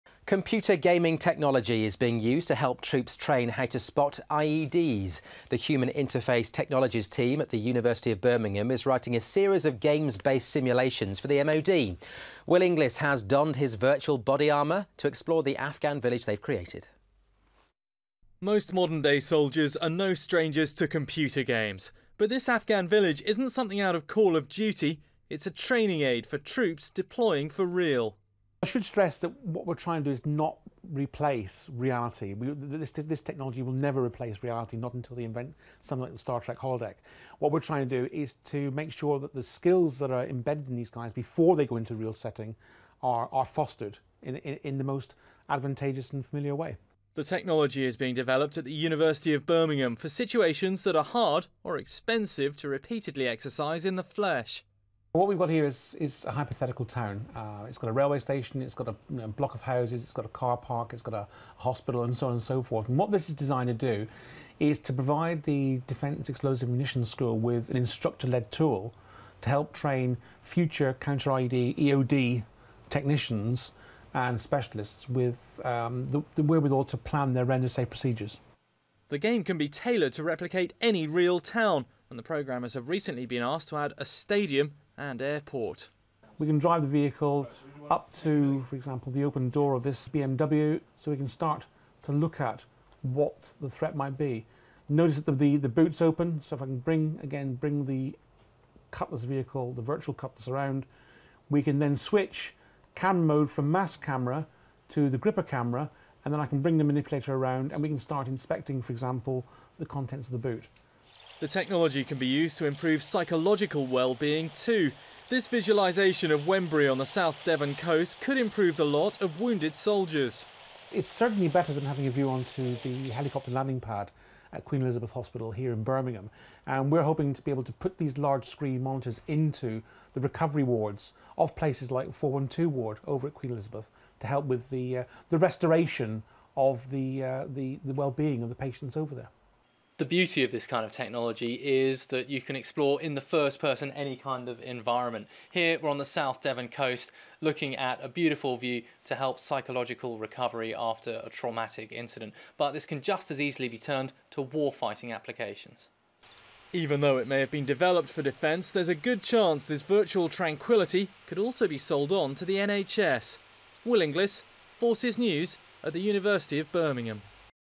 Computer game technology gives troops edge 29.07.11
Computer game technology is being used to help troops train how to spot IEDs. The human interface technologies team at the University of Birmingham is writing a series of games based simulations for the Ministry of Defence. They include a virtual reality Afghan village and a programme to train bomb disposal experts how to react to threats in real life Britain The 'game' can be tailored to replicate any real town and the programmers have recently been asked to add a stadium and airport.